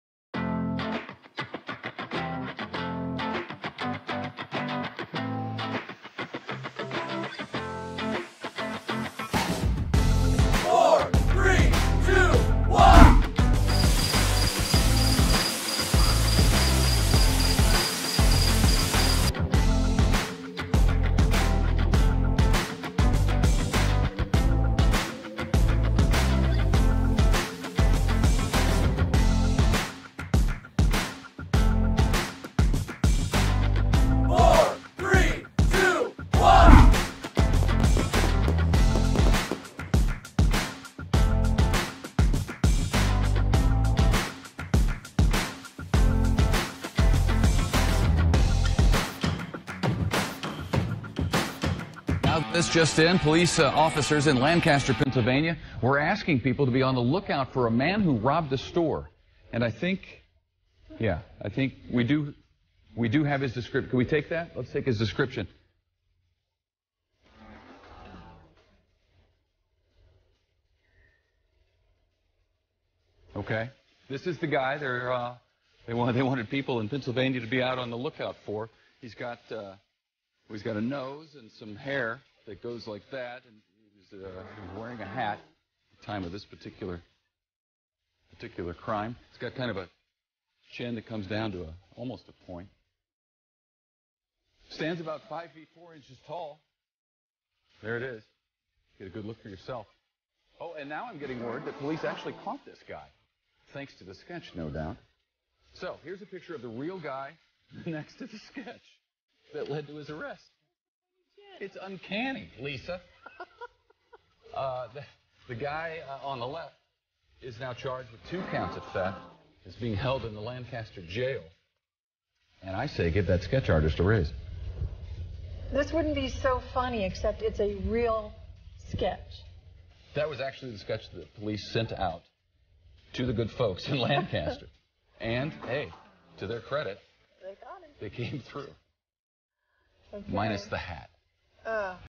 This News Anchors Profiling Will Make Your Day!
🎥 Watch as seasoned professionals stumble over their words, face unexpected technical glitches, and find themselves in the most unexpected and amusing situations. From tongue-tied slip-ups to unexpected outbursts of laughter, these bloopers will leave you in stitches! 🤭💥

👉🏽 Join us on this light-hearted journey through the world of news reporting, where even the most serious of topics can turn into moments of pure comedy gold! From unexpected mishaps to comical interactions with guests, these bloopers showcase the human side of journalism. 🎙️🙈

💡 Our intention is not to poke fun at these talented professionals, but to spread some much-needed joy and lightheartedness in today's busy world. 🌟🌈

📢 Tag someone who loves a good laugh and let us know your favorite blooper moment in the comments below! 🤳🔔